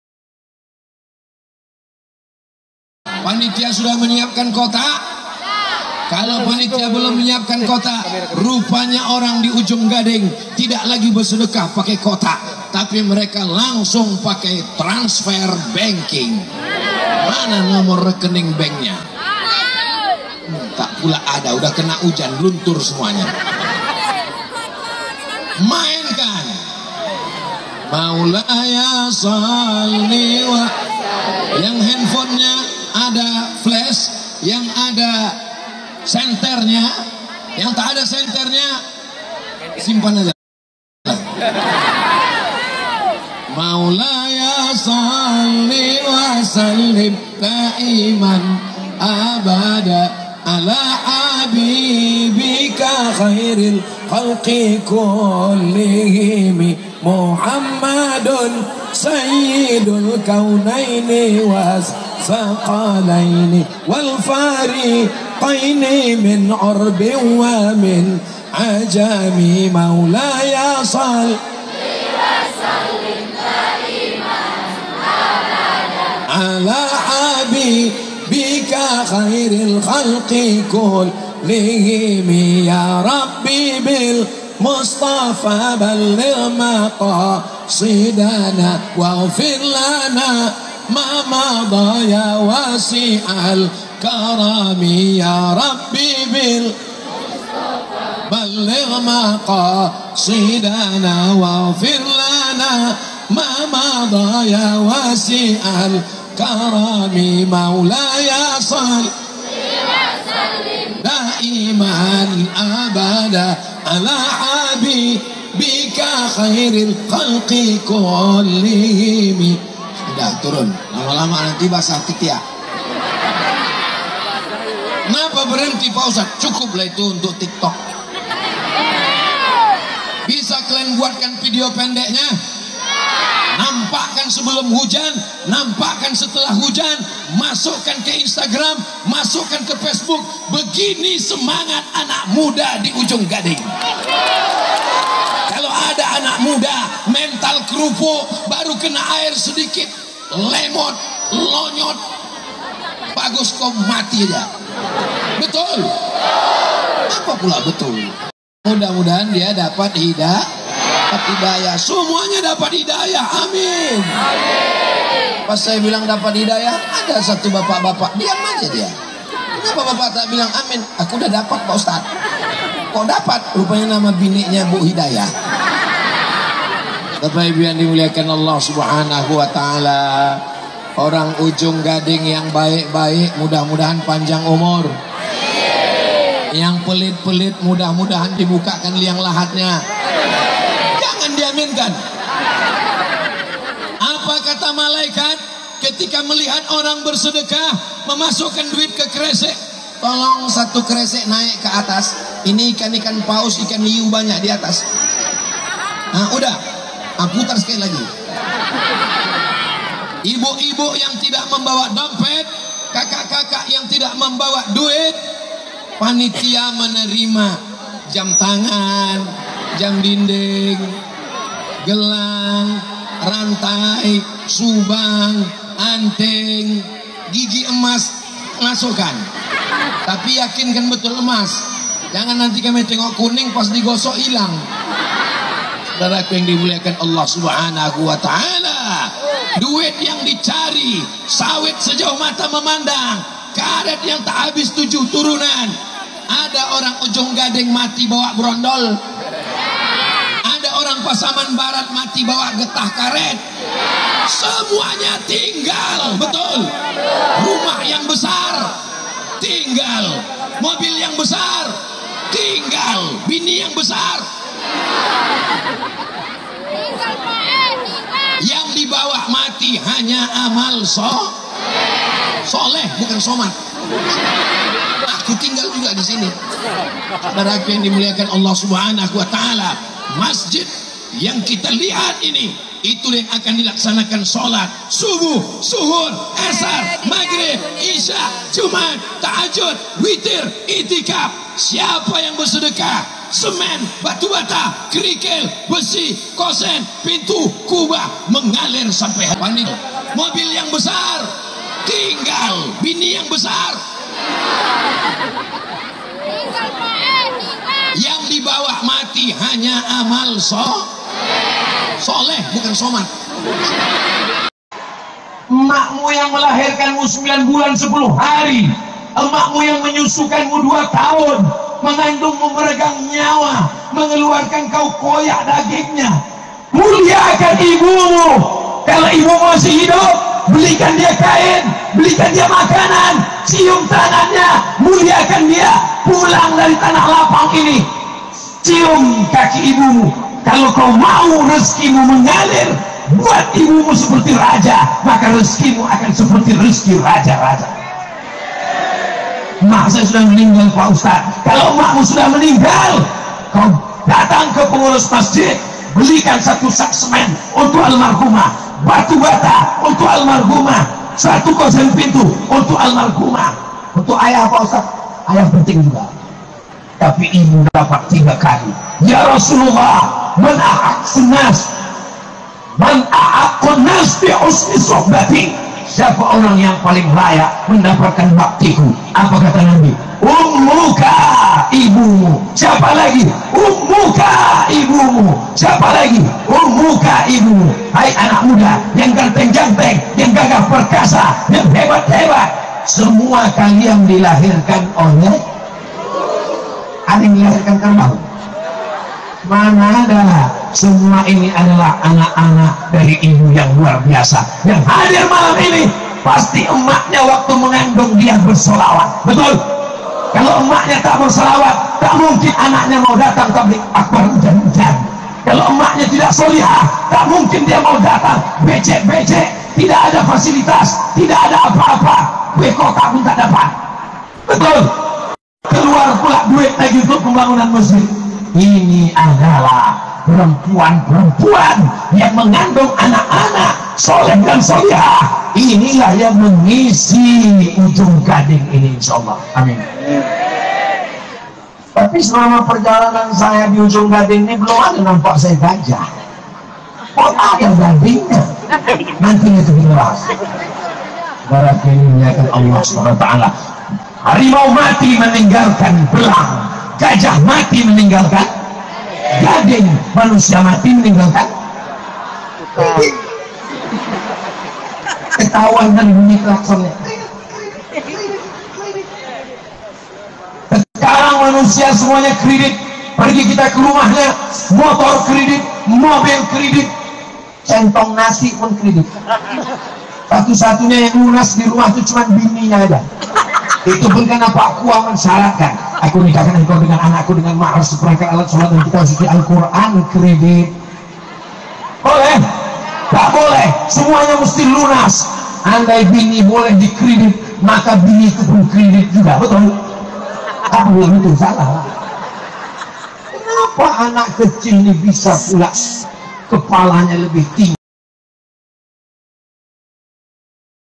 Ustadz Abdul Somad di Ujung Gading Pasaman Barat
Ustadz Abdul Somad Tabligh Akbar di Lapangan Kuamang Ujung Gading Pasaman Barat, minggu malam 28 Mei 2023.